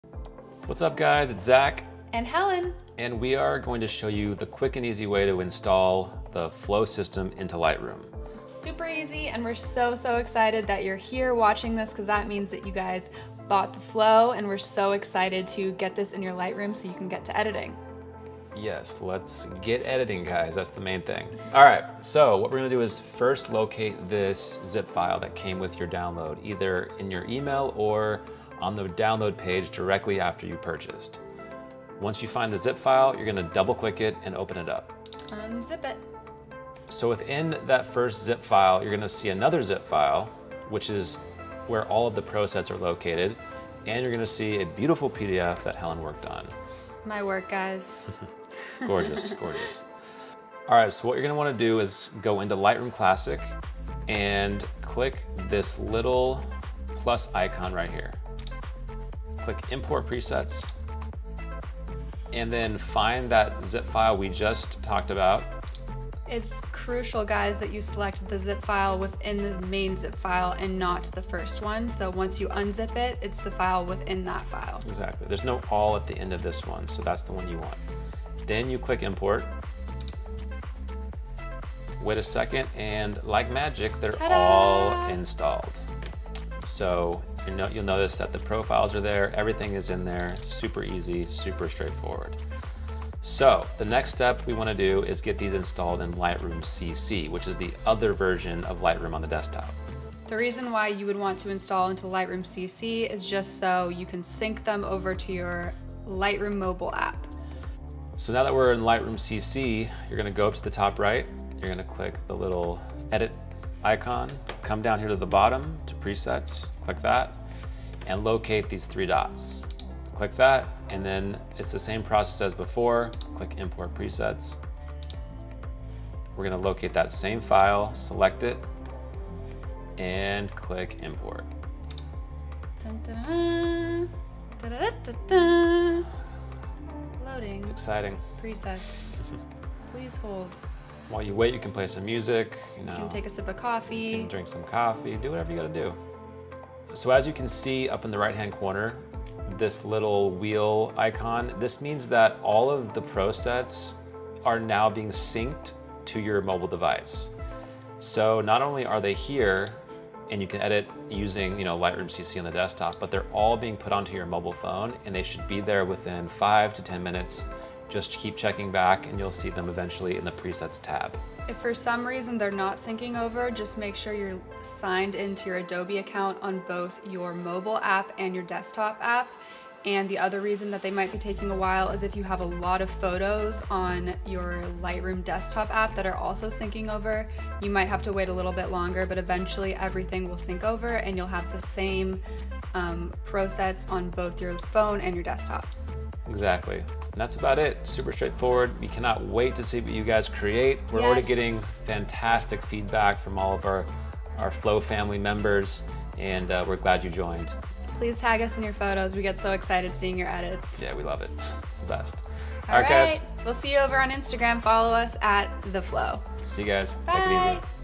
How To Install Phlow (Presets) Into Lightroom | Best and Easiest Way!
Hi and thanks for downloading The Phlow! And if you're just looking for preset importing help, we hope you find this video useful. This is by far the easiest way to install Phlow Prosets (Presets) into Lightroom for Desktop and Mobile!